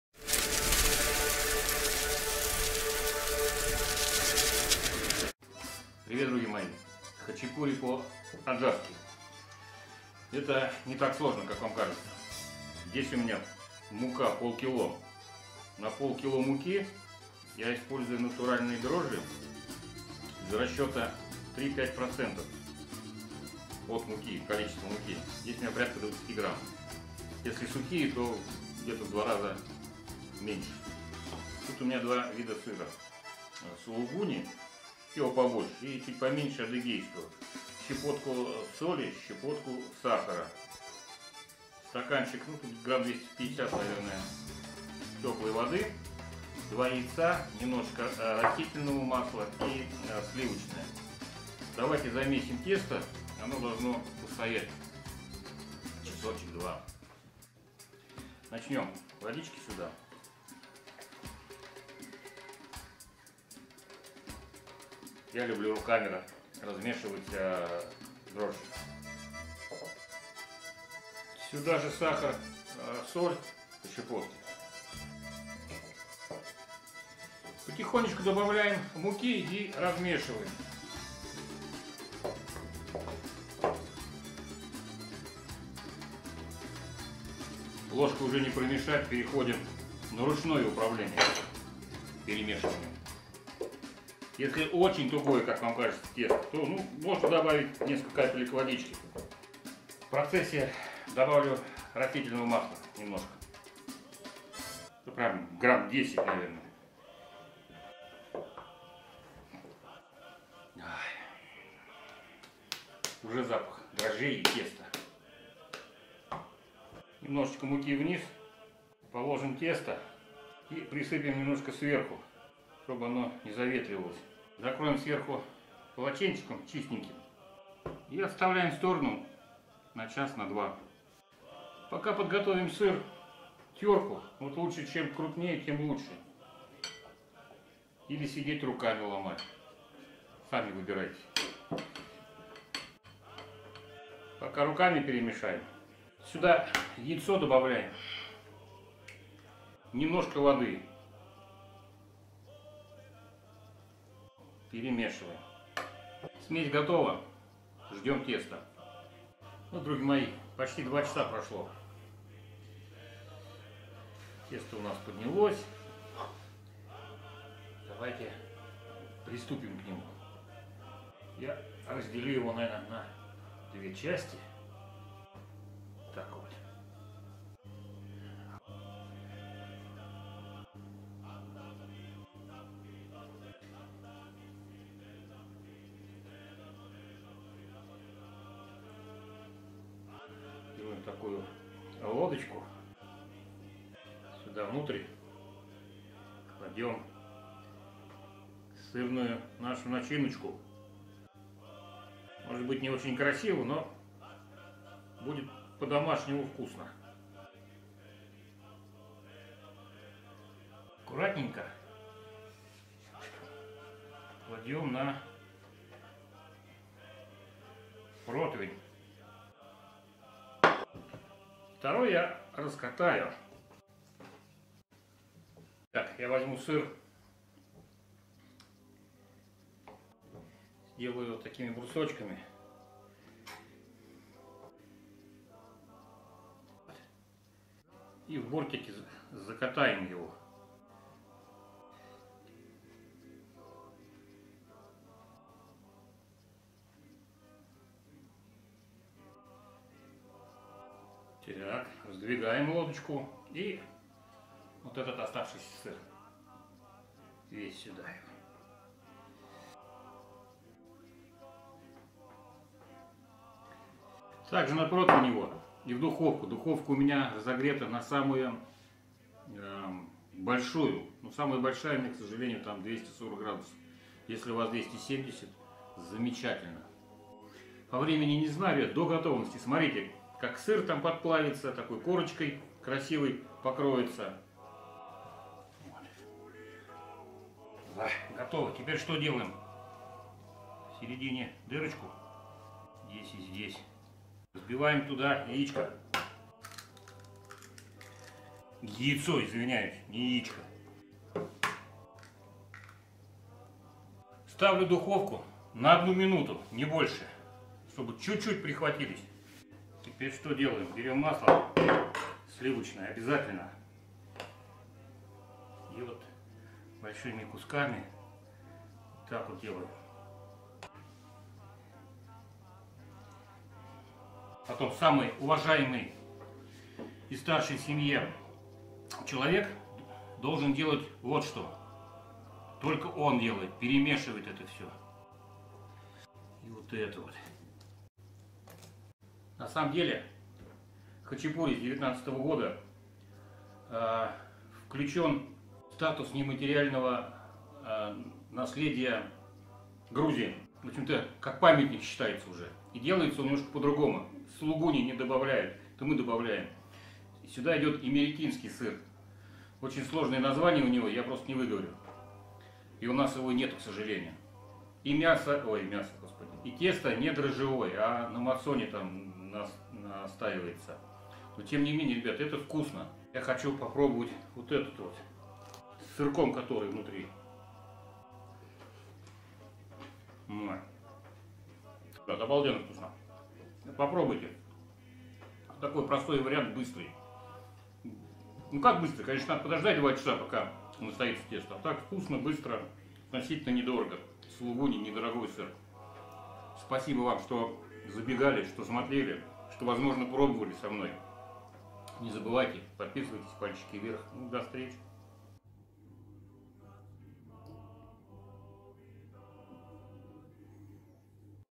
ХАЧАПУРИ ПО АДЖАРСКИ ხაჭაპური
Хачапу́ри — блюдо грузинской кухни, грузинское национальное мучное изделие, пирожок с начинкой из сыра и яйца. В 2019 году хачапури присвоен статус нематериального памятника культурного наследия Грузии.
Единого рецепта хачапури не существует — в каждом регионе страны блюдо готовят по-своему.

Мука 500гр
Дрожжи 20гр
Сыр сулугуни 300гр
Сыр адыгейский 200гр
Соль 3гр
Сахар 3гр
Яйцо 3шт
Сливочное масло